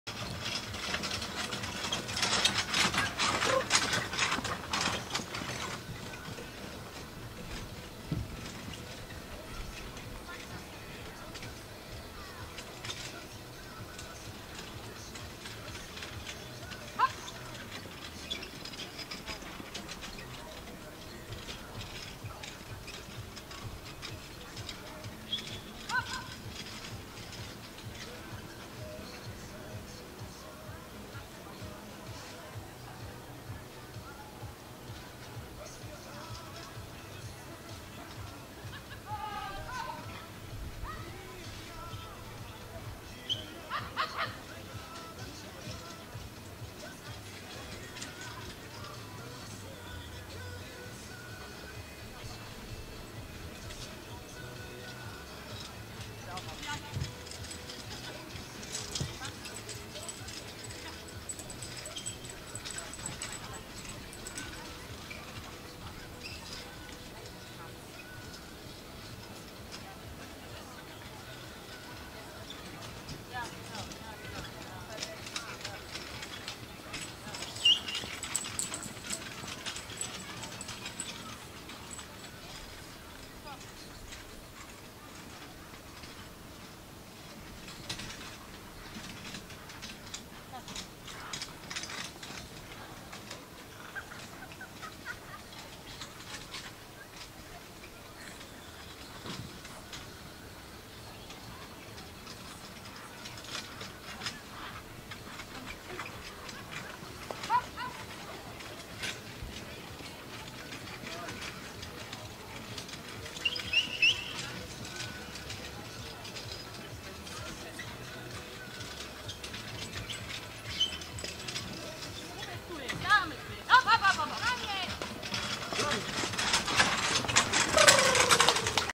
Fahrturnier Lienen (WBO) 8.10.2023
Ein kurzes Video einer Kegel-Fahr-Prüfung mit festen Gelände Hindernissen vom Fahrturnier in Lienen
WBO
Fahrertag
Bilder von verschiedenen Turniergelände bei Instagram:
Pferde.Flo